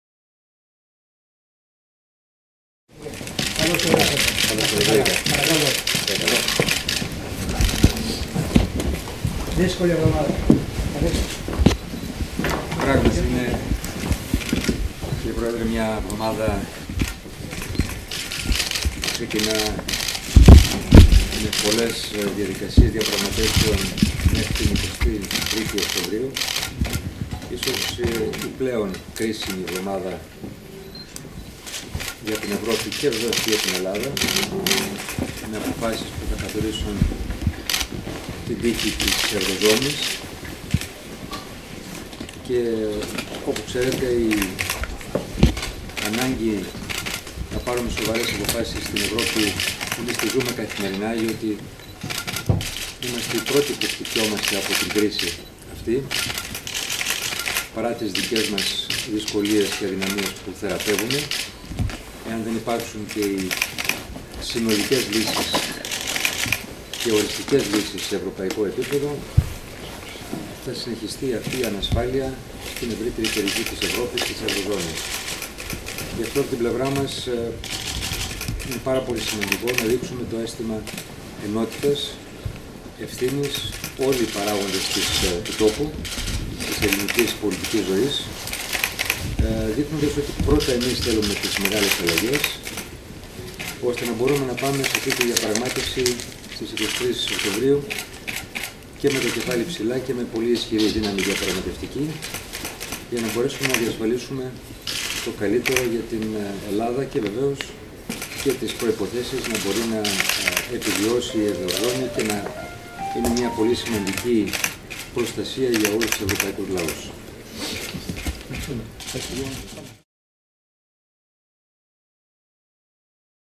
Συνάντηση με τον Πρόεδρο της Δημοκρατίας | 17.10.2011
Η συνάντηση του Πρωθυπουργού, Γιώργου Α. Παπανδρέου, με τον Πρόεδρο της Δημοκρατίας Κάρολο Παπούλια.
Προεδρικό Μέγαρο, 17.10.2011